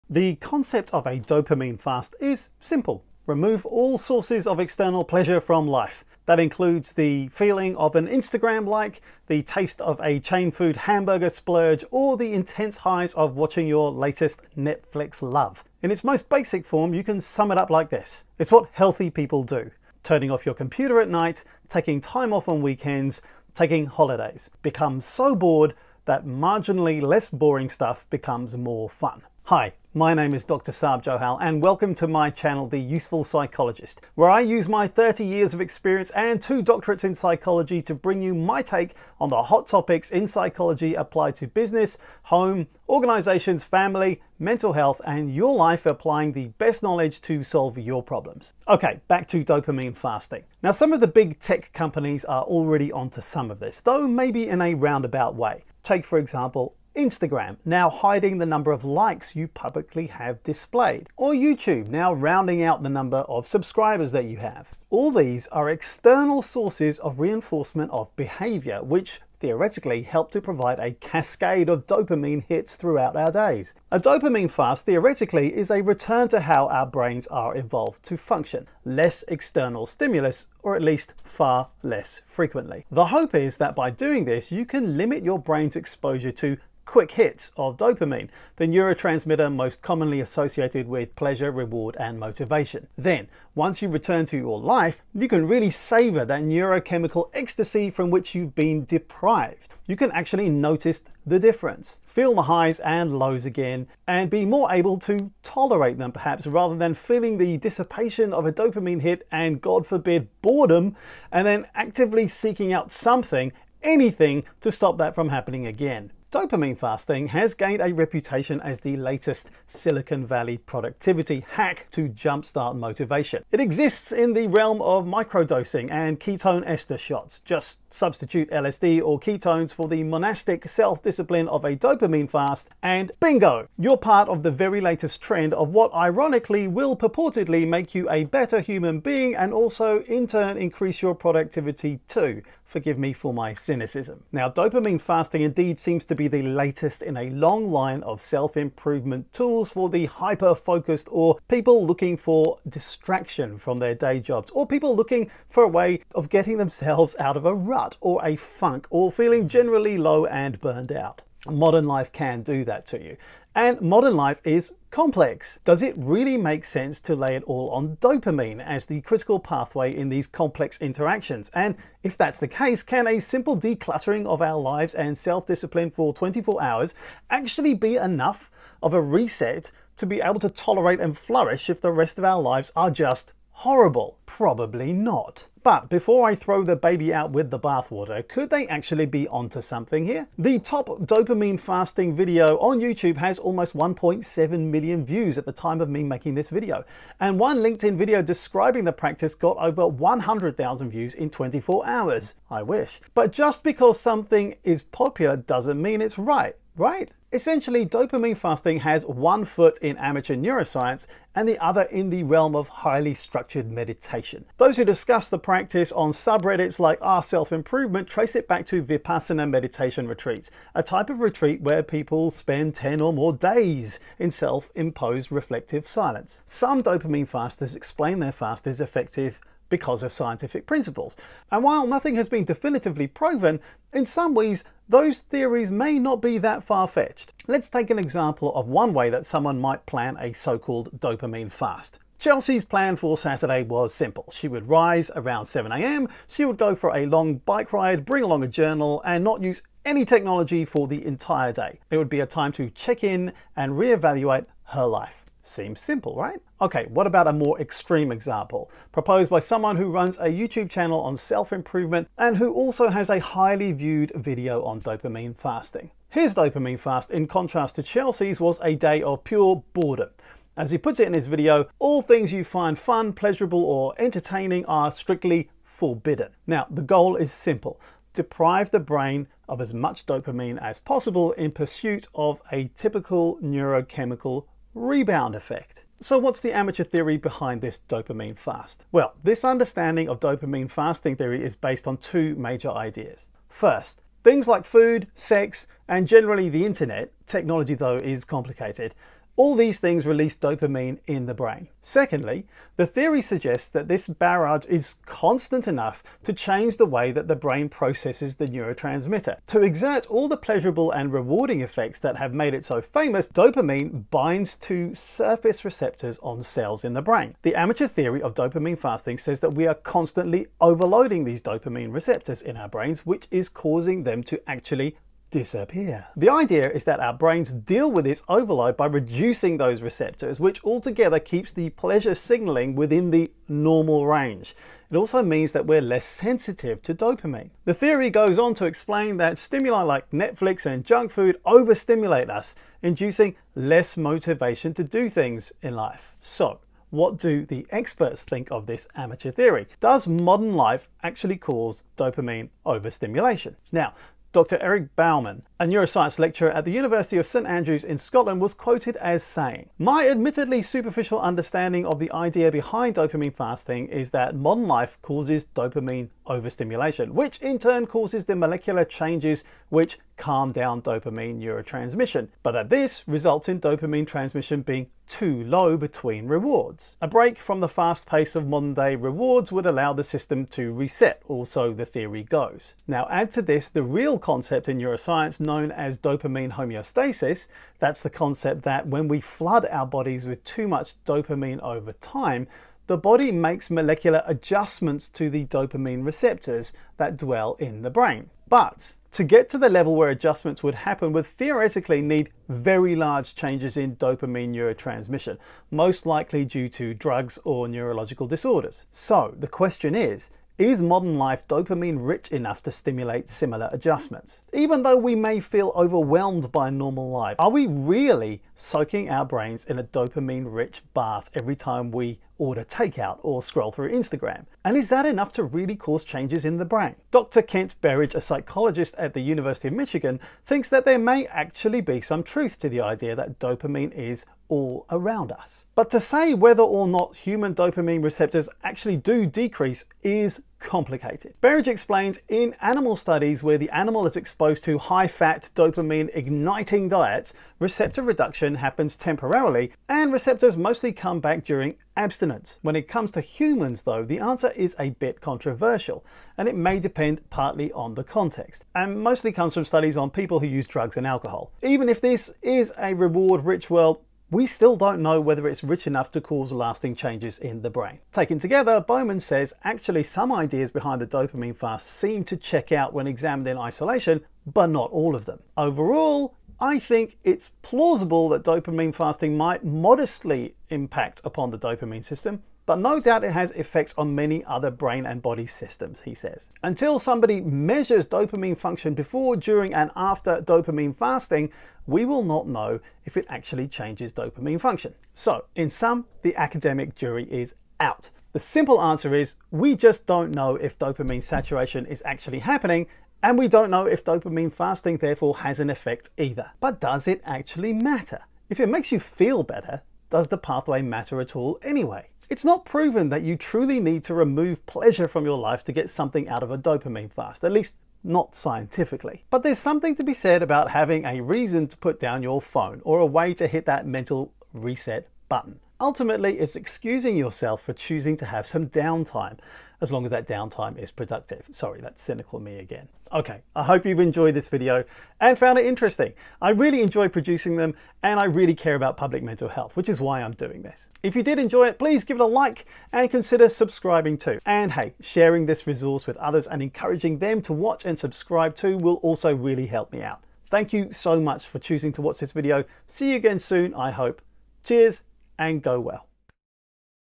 Dopamine Fasting & Your Brain - How Does it Work?
The concept of a dopamine fast is simple: remove all sources of external pleasure from life. That includes the feeling of an Instagram like, the taste of a chain food hamburger splurge, or the intense highs of watching your latest Netflix love. In its most basic form you can sum it up like this: it’s what healthy people do - turning your computer off at night, taking time off on weekends, taking holidays. And the real nuts and bolts down to the basics summary might be: become so bored that marginally less boring stuff becomes fun. Dopamine detox has gained a reputation as the latest Silicon Valley productivity hack to jumpstart motivation. Find out if it has any basis in science, how it might work, and how you might use it in your life.